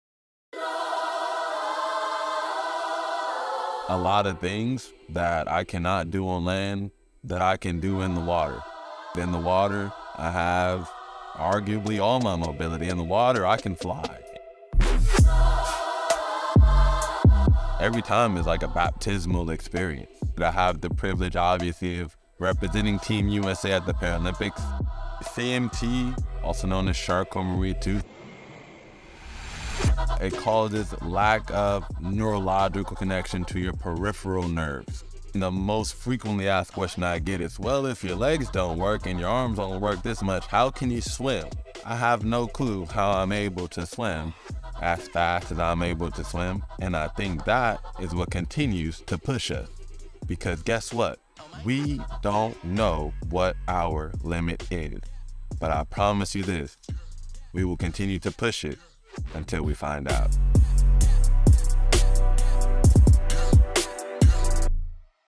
Jamal Hill Olympic Swimmer - Swim up Hill
Delving into the inspiring journey of Jamal Hill, we crafted a short documentary that chronicles his rise as a USA Paralympic swimmer. Our production spanned several days, capturing candid moments both on location and in-studio. In partnership with Hugh Foster, we employed state-of-the-art equipment, from underwater camera rigs and ultra slow-motion capture to advanced cinema robots, ensuring that every stroke, emotion, and triumph of Jamal's story was portrayed in breathtaking detail.

Shot and Edited by Dark Brothers Creative Marcus Sotelo

amal Hill, USA Paralympic Swimmer, Short Documentary, Hugh Foster, Underwater Camera Rigs, Ultra Slow-motion, Cinema Robot.